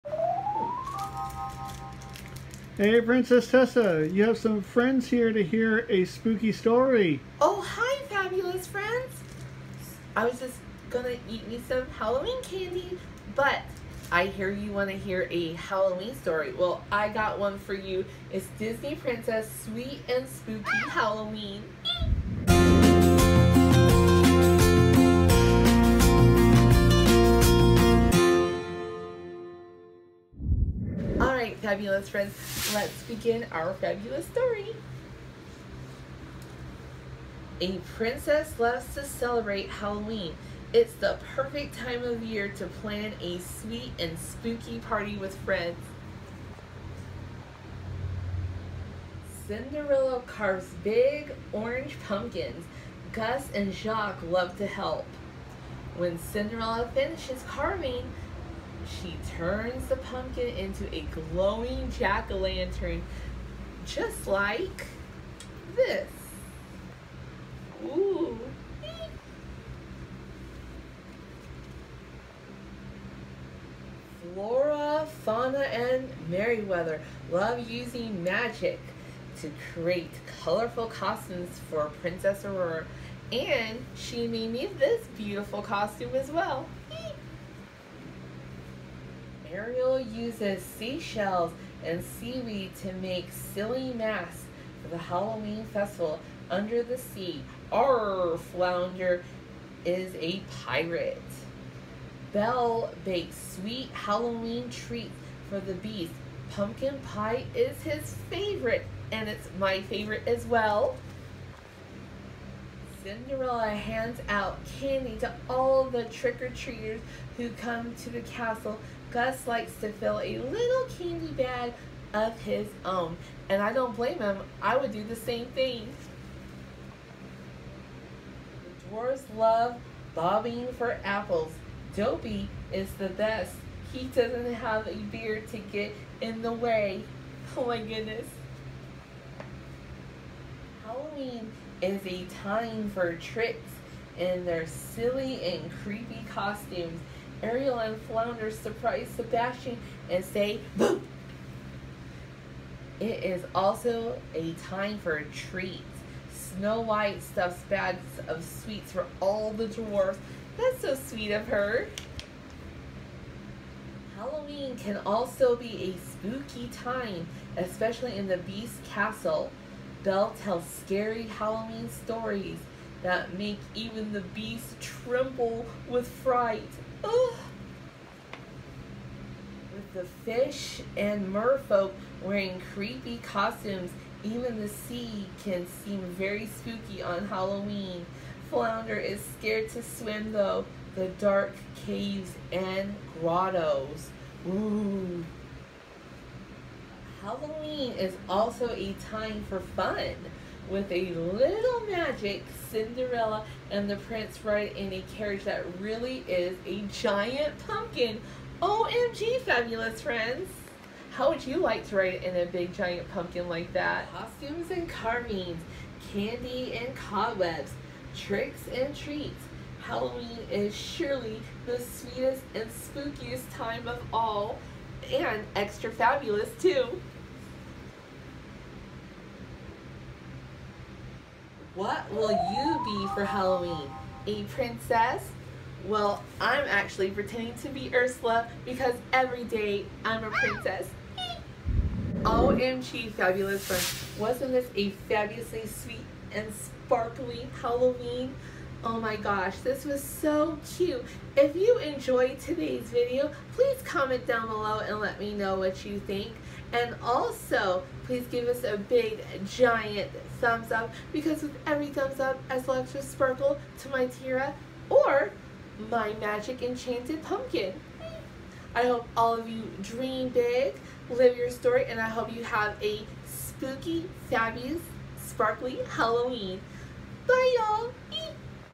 Sweet & Spooky Halloween - Disney Princess 2021 | Story Time | Princess Tessa
Hello fabulous friends, Happy Fabulously sparkly Halloween. Today’s fabulous adventure, Princess Tessa reads her new book Disney Princess Sweet & Spooky Halloween 2021 come read along with us